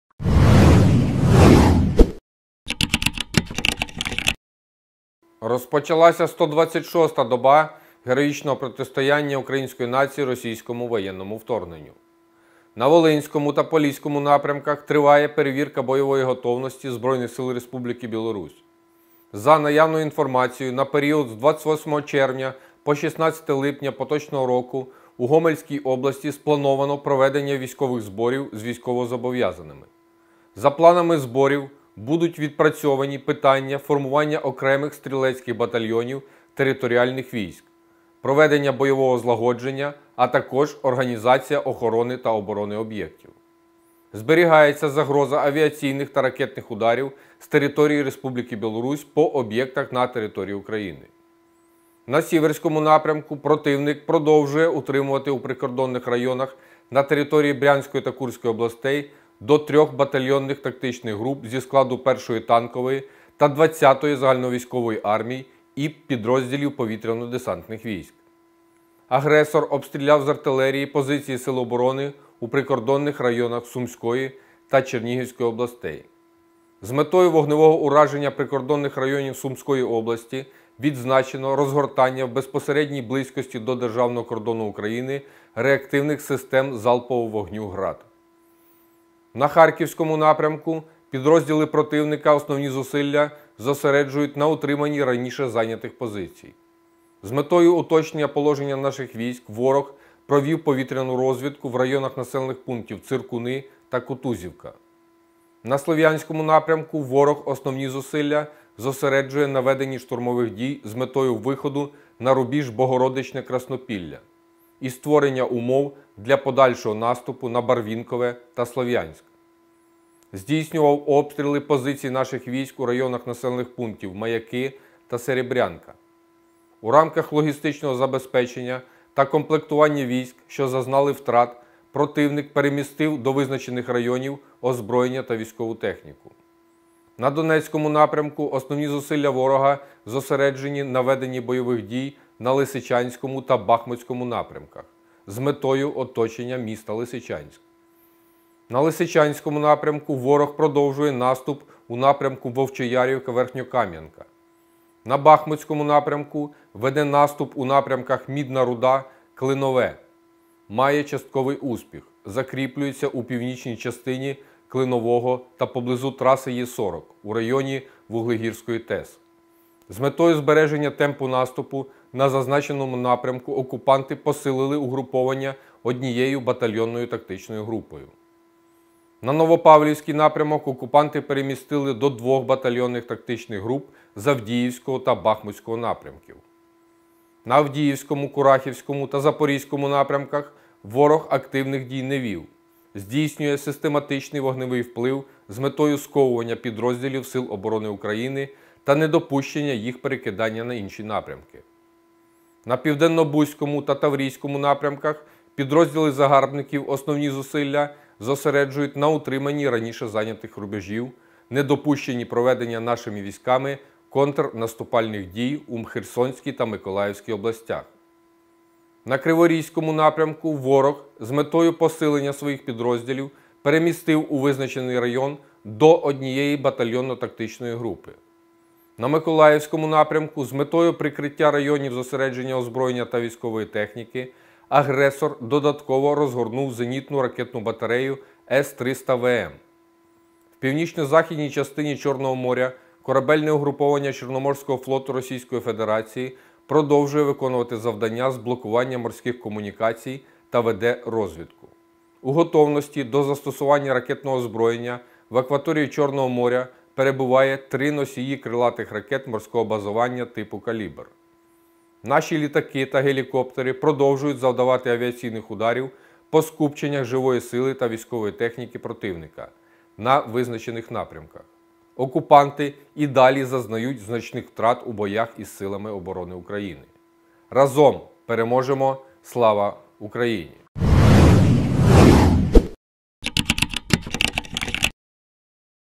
Оперативна інформація станом на 06.00 29.06.2022 щодо російського вторгнення
Розпочалася сто двадцять шоста доба героїчного протистояння Української нації російському воєнному вторгненню.

На Волинському та Поліському напрямках триває перевірка бойової готовності збройних сил республіки білорусь. За наявною інформацією, на період з 28 червня по 16 липня поточного року у Гомельській області сплановано проведення військових зборів з військовозобов’язаними. За планами зборів будуть відпрацьовані питання формування окремих стрілецьких батальйонів територіальних військ, проведення бойового злагодження, а також організація охорони та оборони об’єктів.
Зберігається загроза авіаційних та ракетних ударів з території республіки білорусь по об’єктах на території України.

На Сіверському напрямку противник продовжує утримувати у прикордонних районах на території  Брянської та Курської областей до трьох батальйонних тактичних груп зі складу 1-ї танкової та 20-ї загальновійськової армій і підрозділів повітряно-десантних військ.  Агресор обстріляв з артилерії позиції Сил оборони у прикордонних районах Сумської та Чернігівської областей.

З метою вогневого ураження прикордонних районів Сумської області відзначено розгортання в безпосередній близькості до державного кордону України  реактивних систем залпового вогню “Град”.

На Харківському напрямку підрозділи противника основні зусилля зосереджують на утриманні раніше зайнятих позицій. З метою уточнення положення наших військ ворог провів повітряну розвідку в районах населених пунктів Циркуни, Кутузівка.

На Слов’янському напрямку ворог основні зусилля зосереджує на веденні штурмових дій з метою виходу на рубіж Богородичне – Краснопілля і створення умов для подальшого наступу на Барвінкове та Слов’янськ.

Здійснював обстріли позицій наших військ у районах населених пунктів Маяки та Серебрянка.

У рамках логістичного забезпечення та комплектування військ, що зазнали втрат, противник перемістив до визначених районів озброєння та військову техніку.

На Донецькому  напрямку основні зусилля ворога зосереджені на веденні бойових дій на Лисичанському та Бахмутському напрямках з метою оточення міста Лисичанськ.

На Лисичанському напрямку ворог продовжує наступ у напрямку Вовчоярівка – Верхньокамʼянка.

На Бахмутському напрямку веде наступ у напрямках Мідна Руда – Клинове. Має частковий успіх, закріплюється у північній частині Клинового та поблизу траси Е-40 (у районі Вуглегірської ТЕС). З метою збереження темпу наступу на зазначеному напрямку окупанти посилили угруповання однією батальйонною тактичною групою.

На Новопавлівський напрямок окупанти перемістили до двох батальйонних тактичних груп з Авдіївського та Бахмутського напрямків.

На Авдіївському, Курахівському та Запорізькому напрямках ворог активних дій не вів. Здійснює систематичний вогневий вплив з метою сковування підрозділів Сил оборони України та недопущення їх перекидання на інші напрямки.

На Південнобузькому та Таврійському напрямках підрозділи загарбників основні зусилля зосереджують на утриманні раніше зайнятих рубежів, недопущенні проведення нашими військами контрнаступальних дій у Херсонській та Миколаївській областях.

На Криворізькому напрямку ворог, з метою посилення своїх підрозділів, перемістив у визначений район до однієї батальйонної тактичної групи.

На Миколаївському напрямку, з метою прикриття районів зосередження озброєння та військової техніки,  агресор додатково розгорнув зенітну ракетну батарею С-300ВМ.

В північно-західній частині Чорного моря корабельне угруповання чорноморського флоту російської федерації продовжує виконувати завдання з блокування морських комунікацій та веде розвідку. У готовності до застосування ракетного озброєння в акваторії Чорного моря перебуває три носії крилатих ракет морського базування типу “Калібр”.

Наші літаки та гелікоптери продовжують завдавати авіаційних ударів по скупченнях живої сили та військової техніки противника на визначених напрямках. Окупанти і далі зазнають значних втрат у боях із Силами оборони України.
Разом переможемо! Слава Україні!